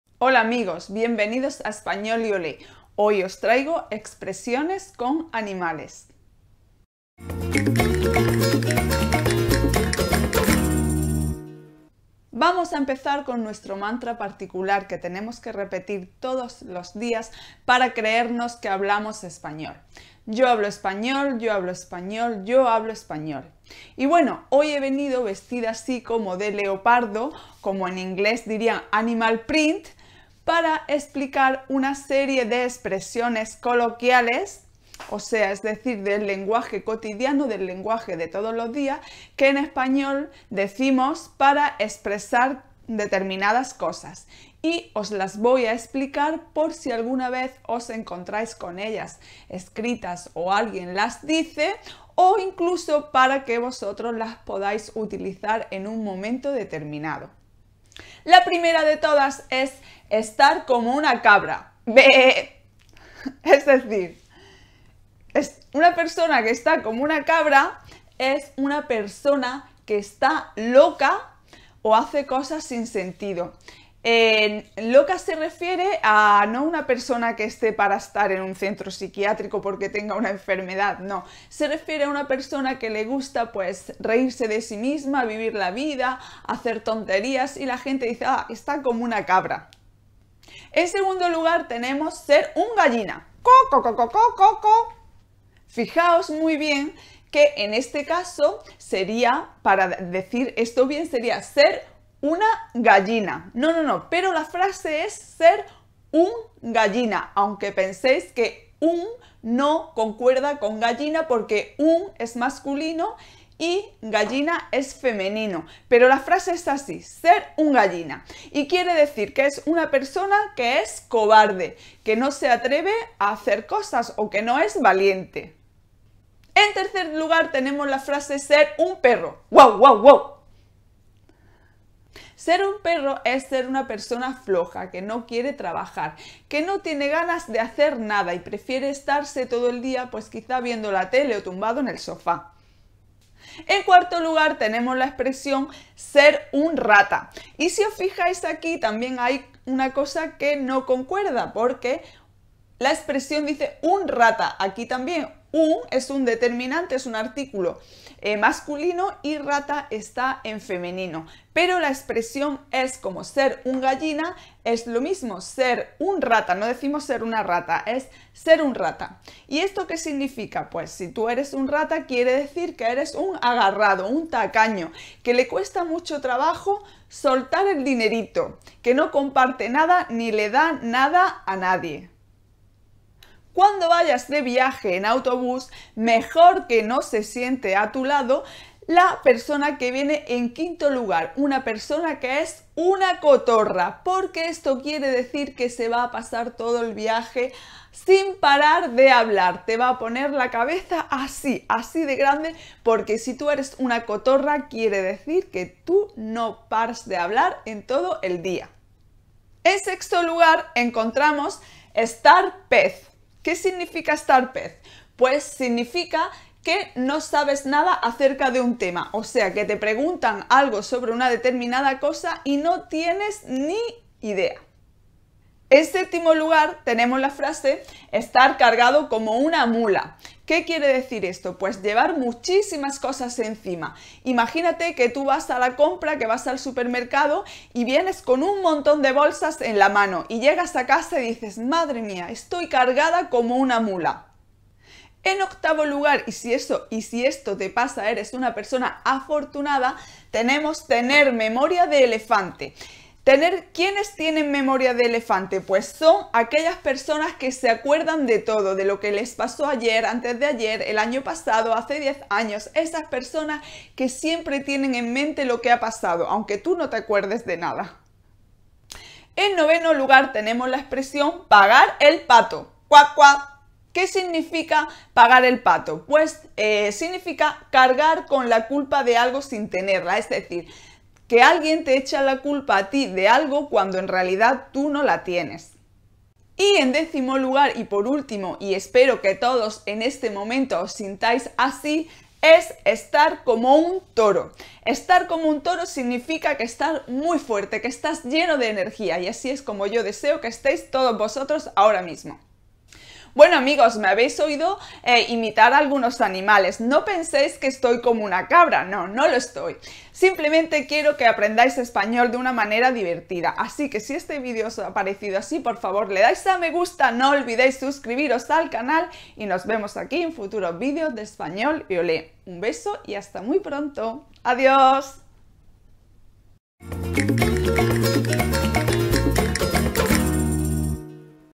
🙈 🐞 FRASES con ANIMALES en ESPAÑOL. 👉 ¡ESTOY COMO UNA CABRA! 👈
APRENDER ESPAÑOL NIVEL INTERMEDIO
LEARN SPANISH
¡Hola amigos! En este vídeo os explico de una manera divertida lo que significan algunas expresiones en las que utilizamos animales. Espero que os divirtáis, que os guste y os sirva para nuestro propósito de aprender español. Besos!!